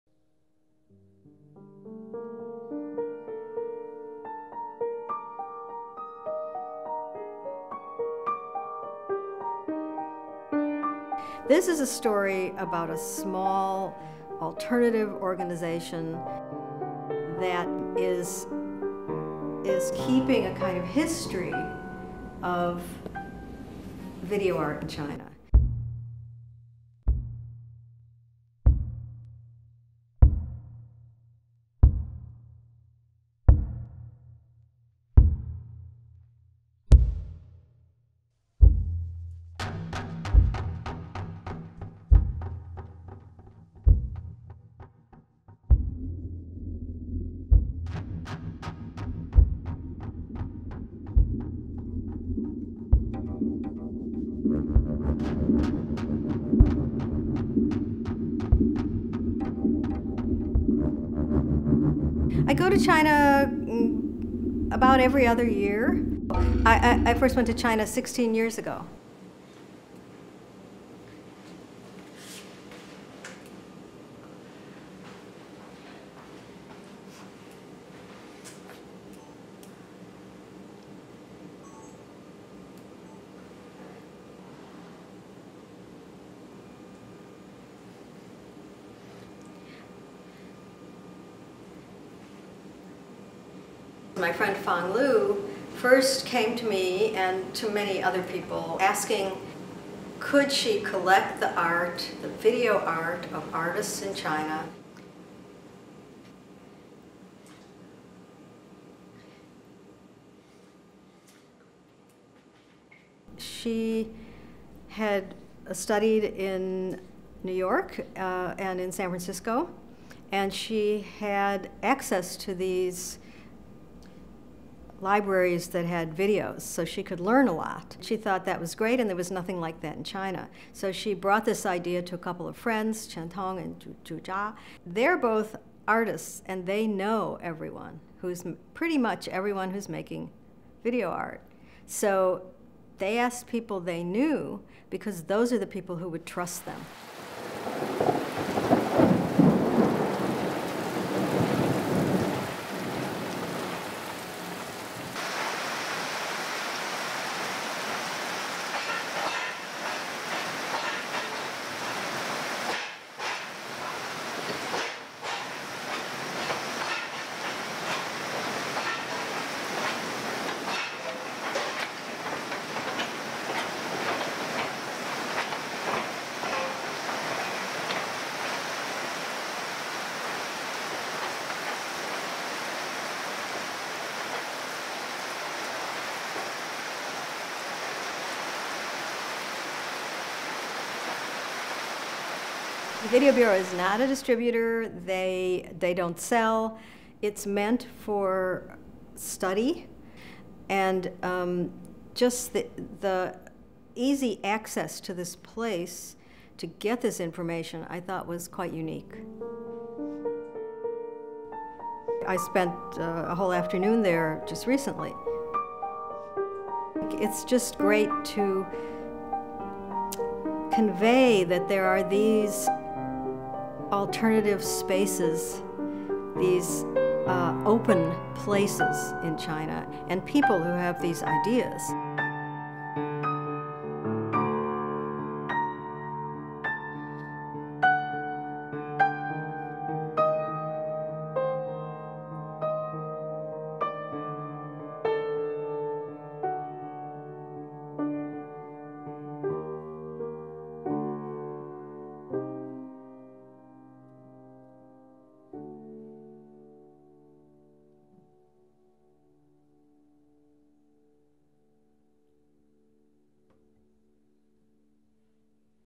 "The Video Bureau of China" from Millennium Film Journal No. 63
Ellen Zweig introduces the Video Bureau, an independent institution in China which collects and archives video art. Ellen's report on the Video Bureau is published in Millennium Film Journal No. 63 (Spring 2016).